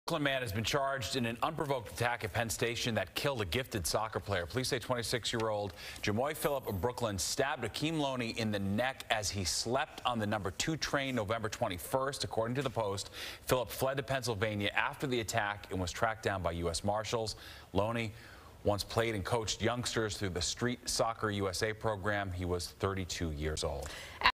Subway stabbing arrest
Jamoy Phillip, 26, was arrested in Pennsylvania by U.S. Marshals for the deadly stabbing of a man on the No. 2 train in November.